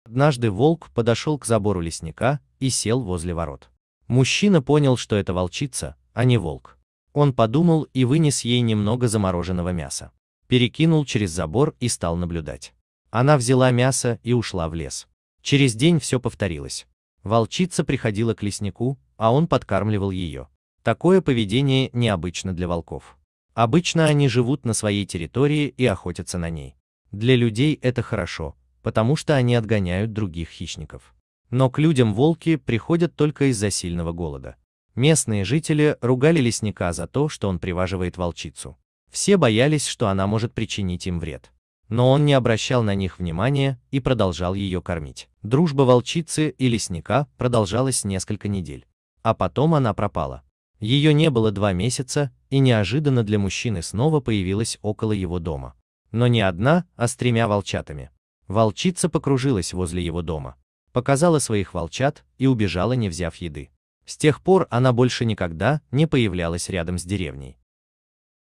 Молодая волчица пришла в деревню и стала просить еду. Лишь один мужчина пожалел её
Однажды волк подошёл к забору лесника и сел возле ворот. Мужчина понял, что это волчица, а не волк. Он подумал и вынес ей немного замороженного мяса. Перекинул через забор и стал наблюдать. Она взяла мясо и ушла в лес.